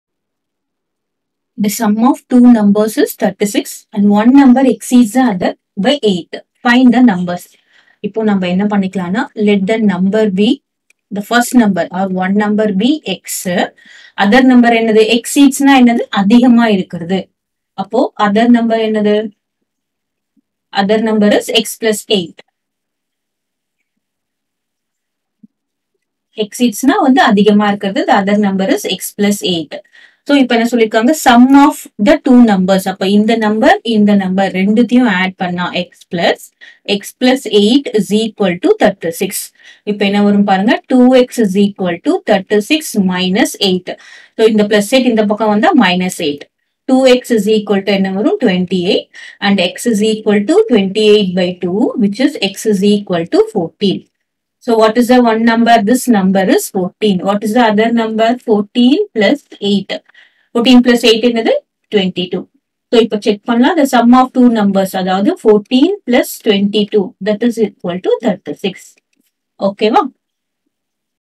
Class 8 TN Maths 3 ALGEBRA Example 3.33 The sum of two numbers is 36 and one number exceeds
Example 3.33
The sum of two numbers is 36 and one number exceeds another by 8.